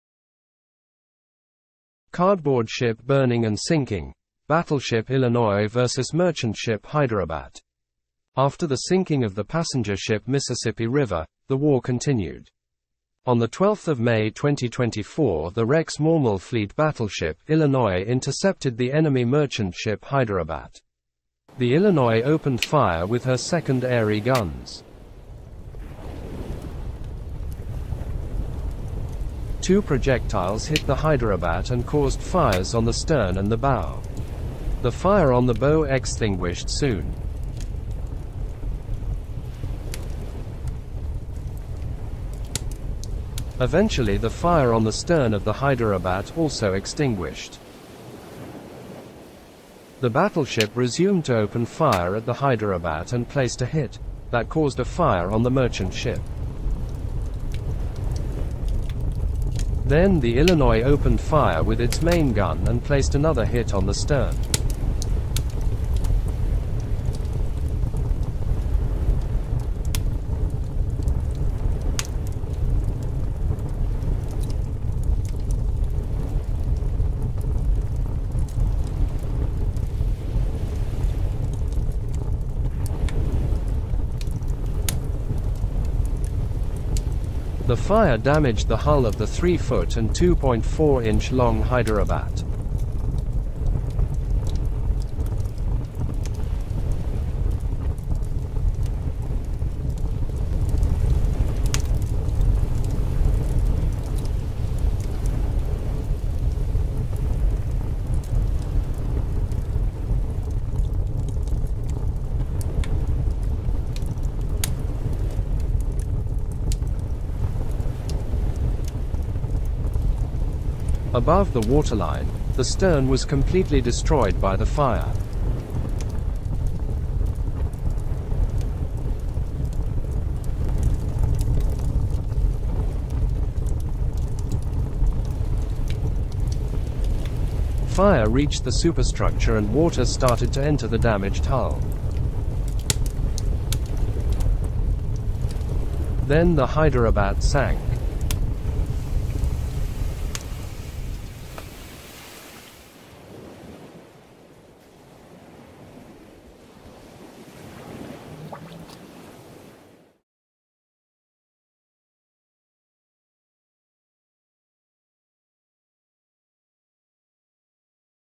Cardboard Ship Burning And Sinking: Battleship Illinois Versus Merchant Ship Hyderabat
After the sinking of the passenger ship Mississippi River, the war continued and on 12 May 2024 the battleship Illinois intercepted the merchant ship Hyderabat and engaged it.

This video is part of The War Of 01 January 2024: On 01 January 2024 the Royal Cardboard Fleet and the RM Fleet declared war on each other. At the start of the war The Royal Cardboard Fleet had the cruiser Hamsire and the battleship Lord Nelson available. Another battleship, the Hindustan was being build and neurally ready for action. The RM Fleet had two cruisers, the Salem and Chester available and the two battleships Kentucky and Illinois.